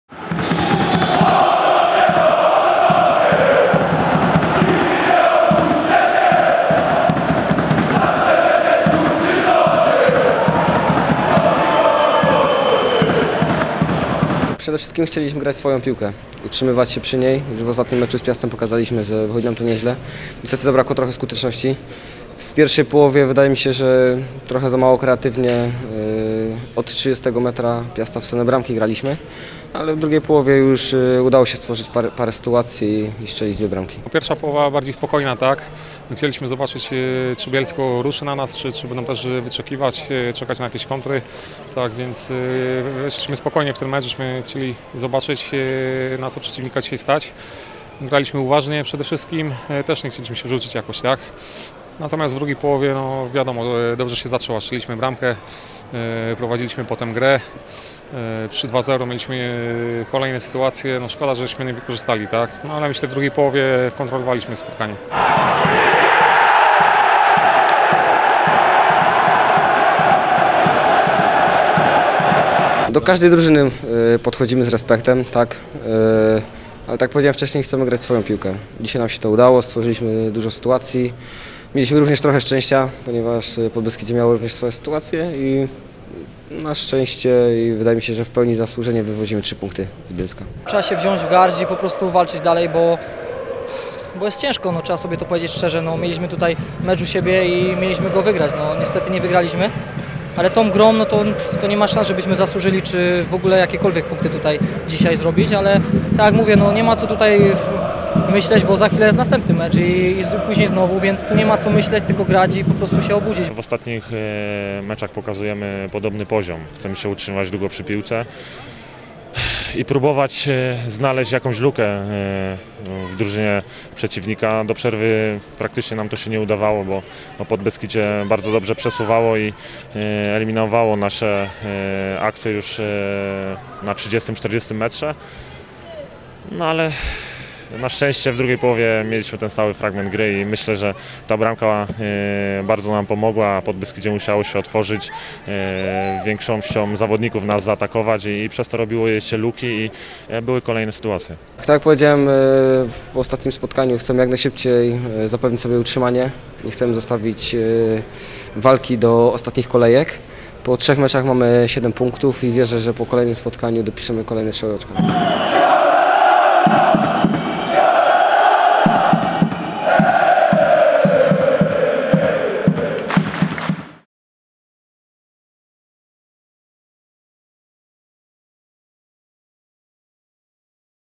TS PODBESKIDZIE 0:2 RUCH
"Niebiescy" wygrywają w Bielsku-Białej po golach Grodzickiego i Gigołajewa.